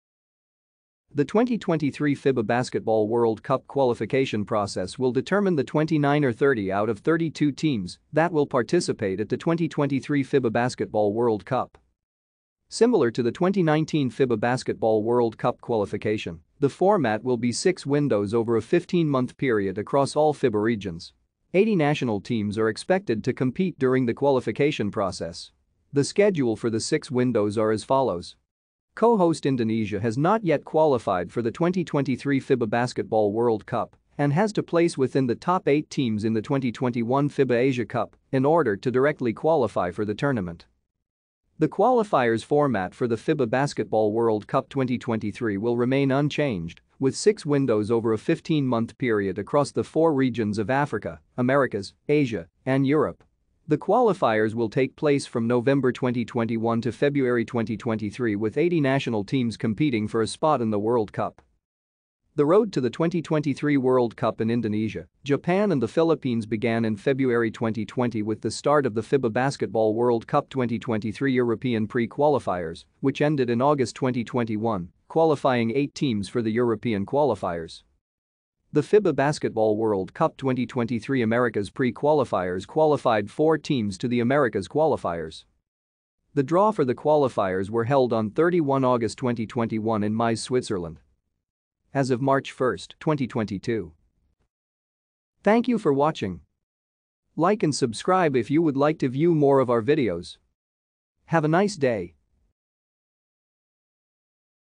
2023 FIBA Basketball World Cup Qualification
The 2023 FIBA Basketball World Cup qualification process will determine the 29 or 30 out of 32 teams that will participate at the 2023 FIBA Basketball World Cup.